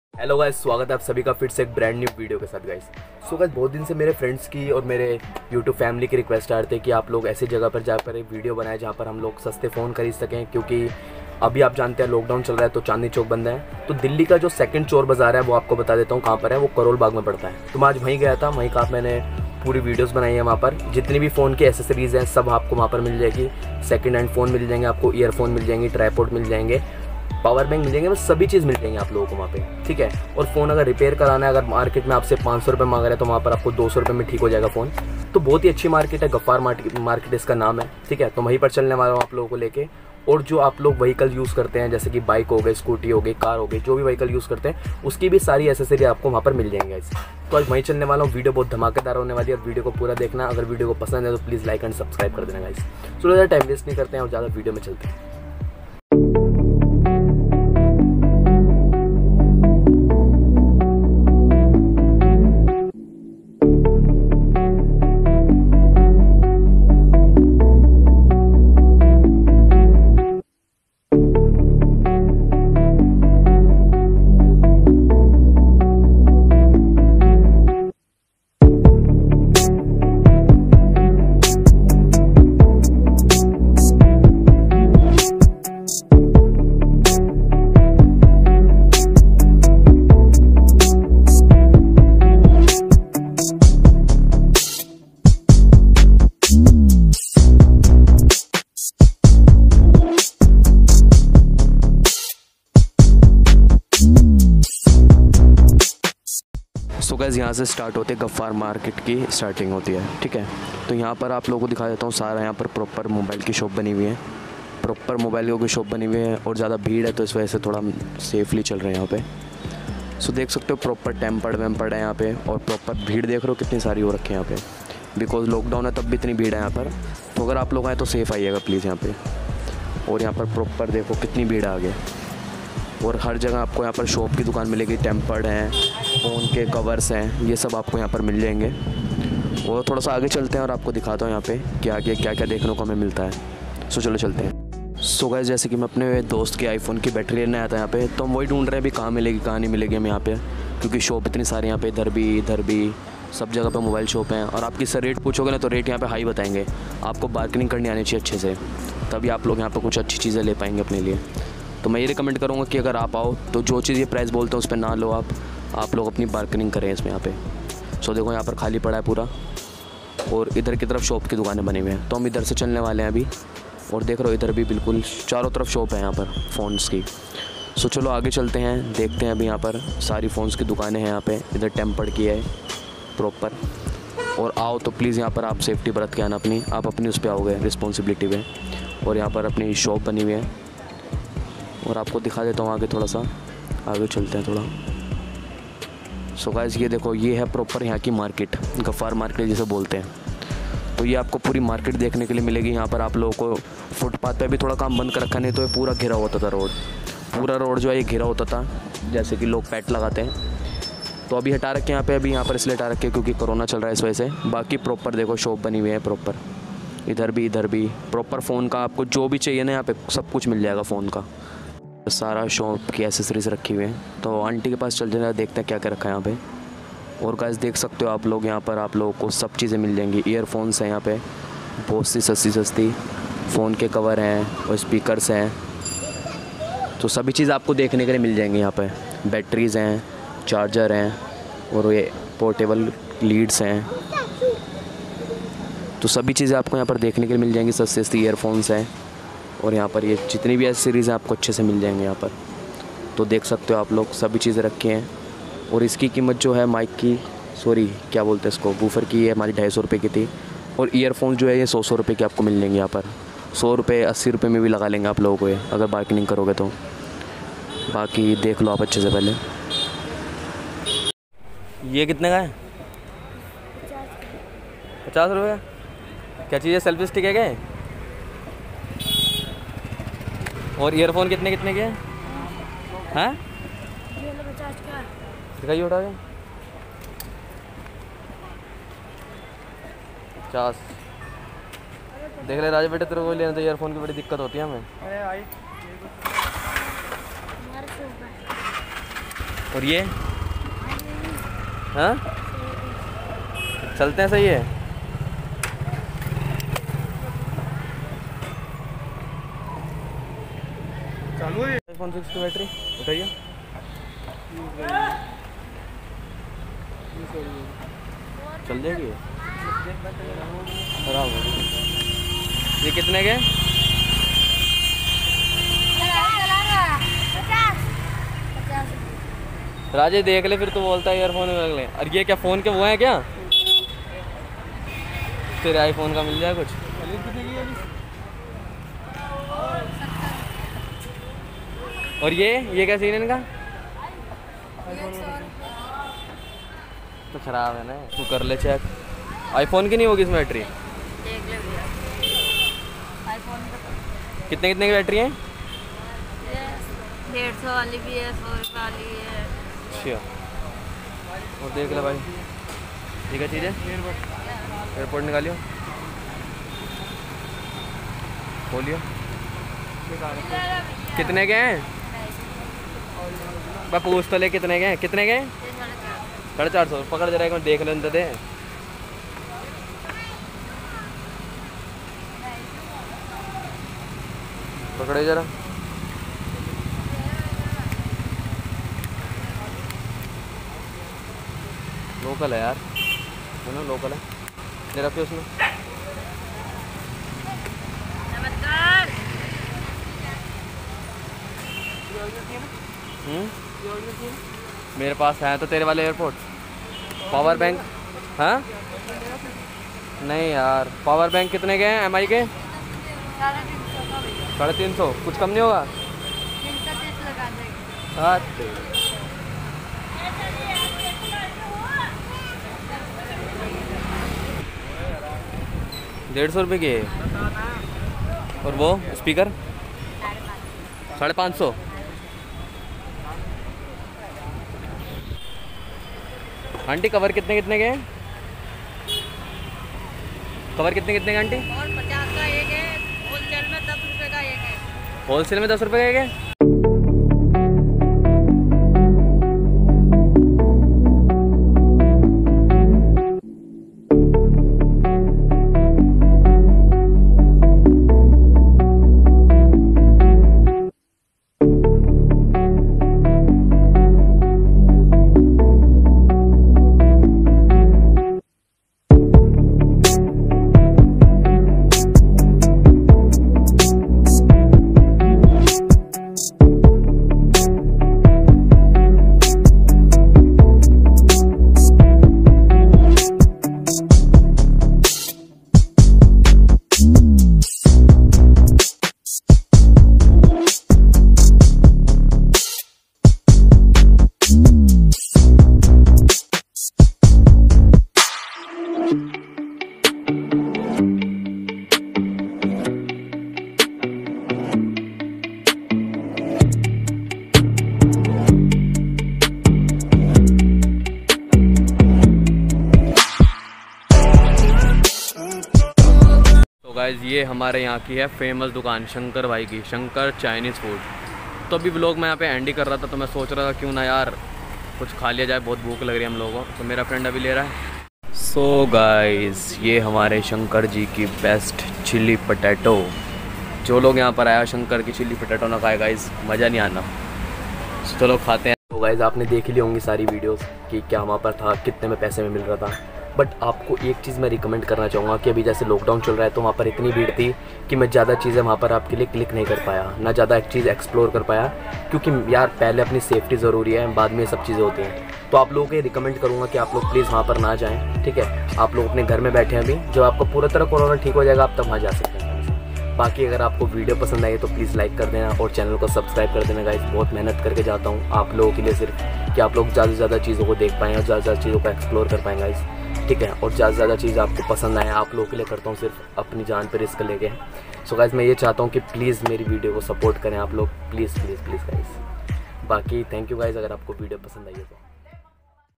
Delhi 2nd Chor bazar🔥 (Gaffar markit) You can buy .phones ,earphones,power bank,batterys .
hey guys welcome back with another video